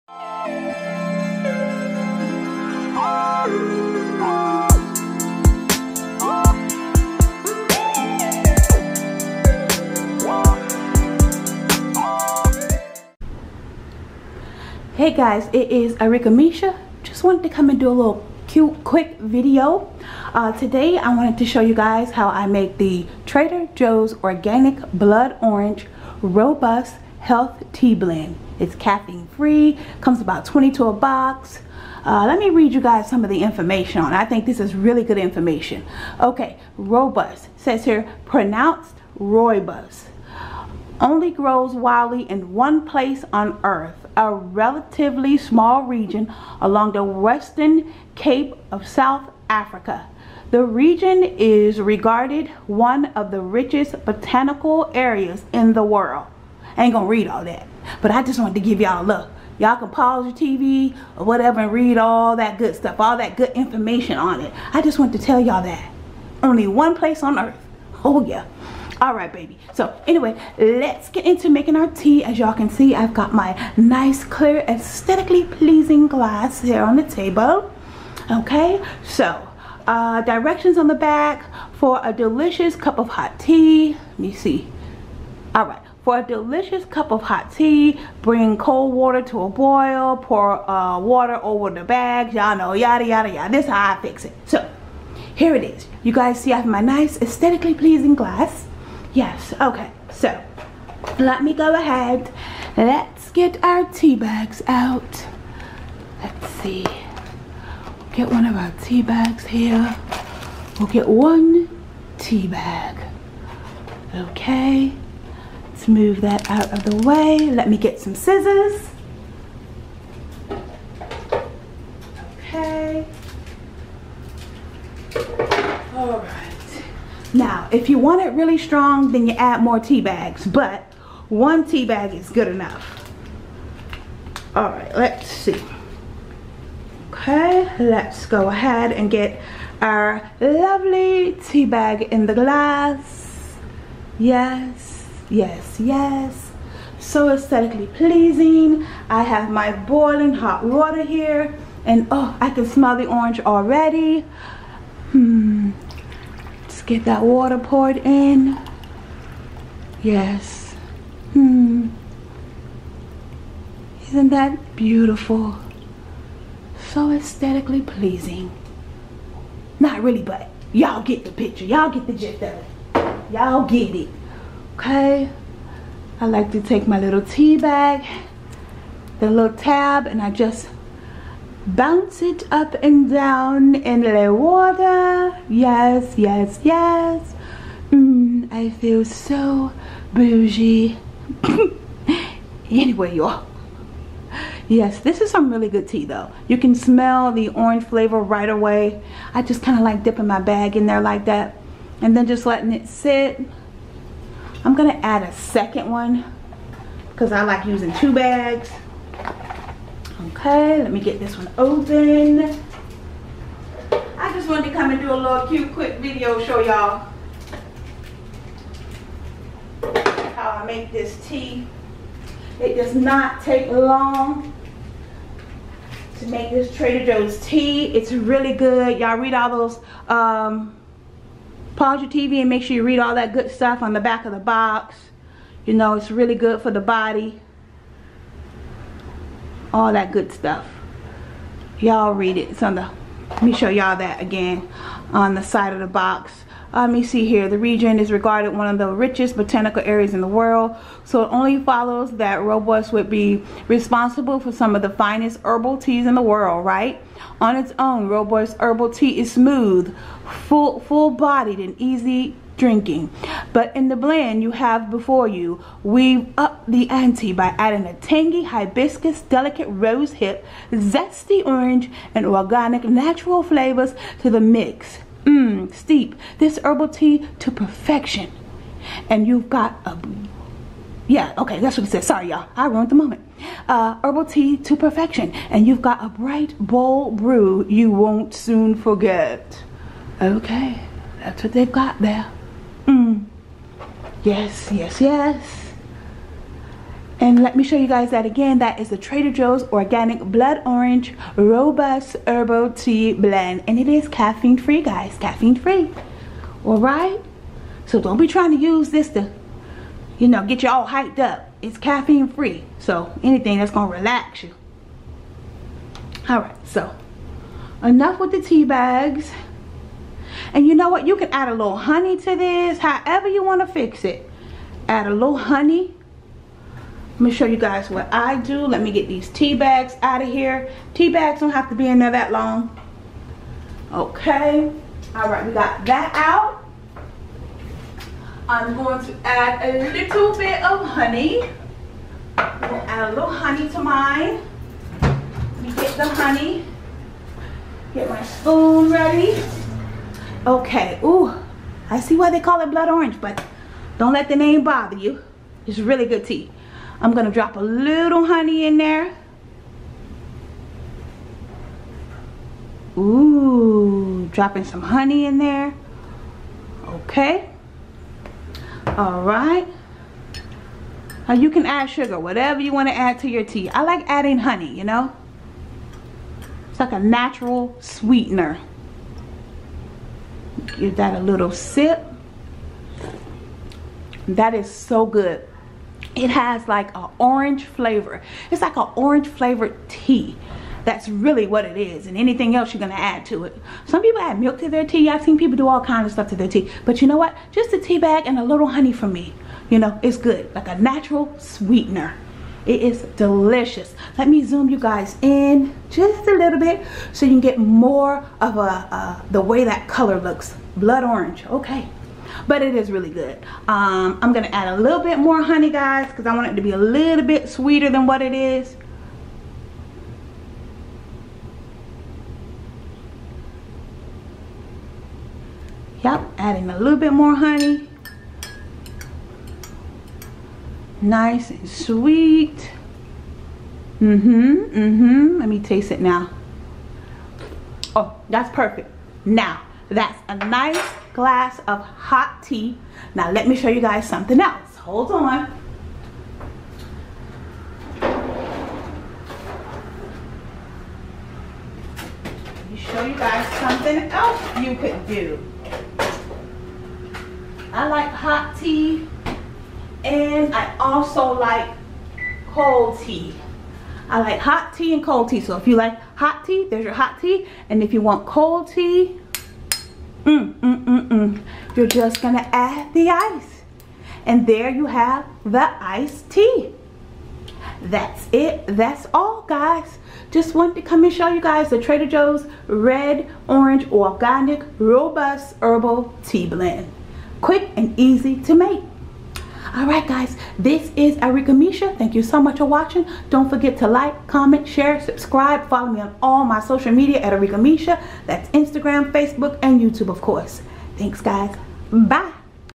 Trader Joe's/Blood Orange Herbal Tea/Arickamisha
This tea is refreshing. I love adding honey to it.